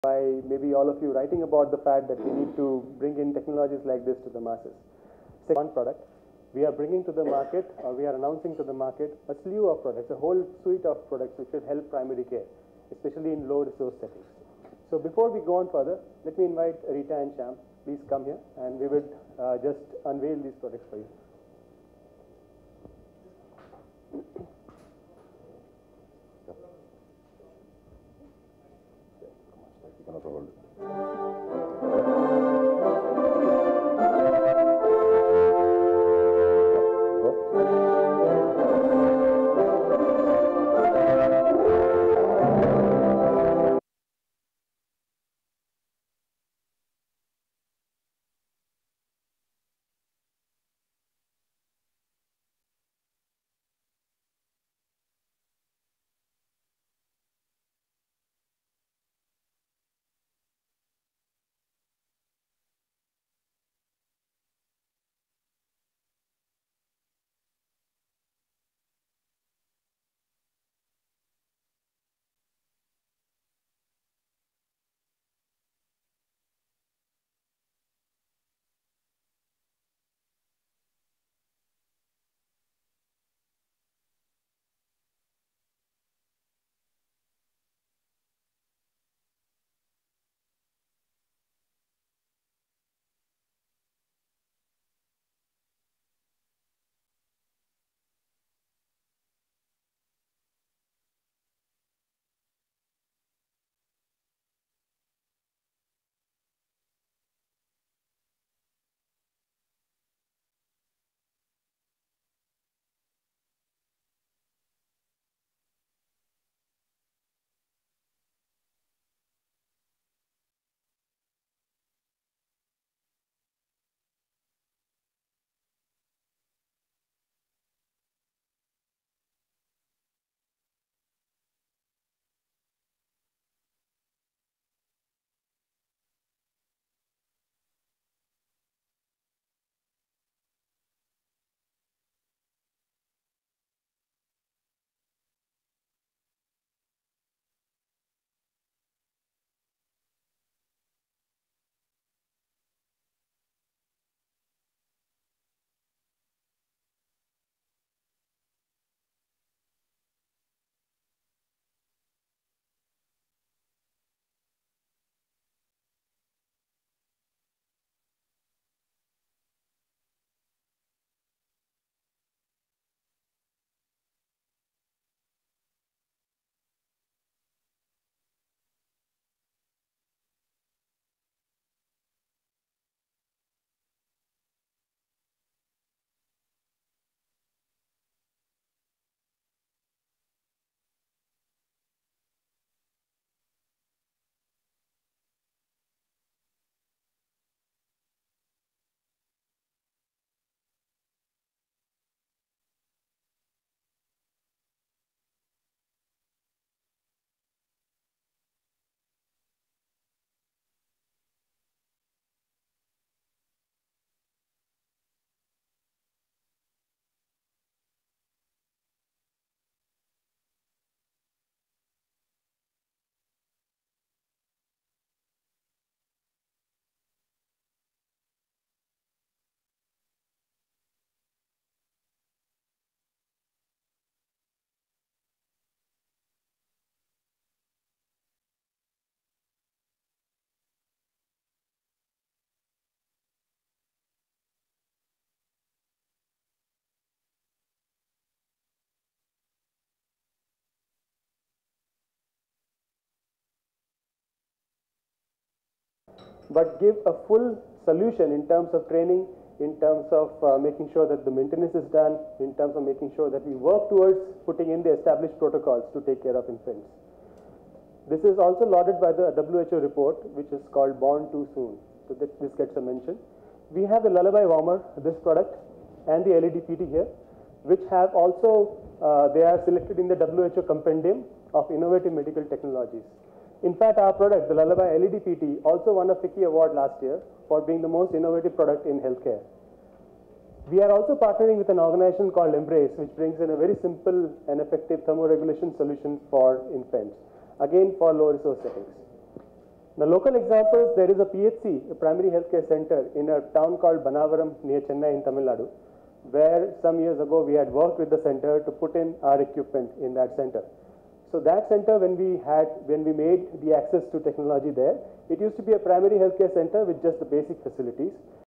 GE Unveils Suite of Solutions for Neonates
GE Healthcare has introduced a suite of new innovative solutions for the survival and growth of neonates in the primary care settings. The new Lullaby Warmer Prime is an easy to use infant warmer that makes safe and reliable thermoregulation accessible for primary care settings even in remote areas. The Lullaby Resus Prime and Lullaby Resus Plus are neonatal resuscitation devices that deliver simple operation with flexible patient circuits etc. The Lullaby suite of infant care solutions are developed in India from ground up with help and inputs from primary care and other healthcare professionals in India. "While the suite of solutions are designed in India for India, they will also benefit the entire world," said Rita M Barksdale, Global General Manager, Maternal Infant Care, GE Healthcare. "These solutions impact quality, are cost-effective and are usable by frontline health workers," said Ashutosh Banerjee, Director of Life Care Solutions, GE Healthcare South Asia.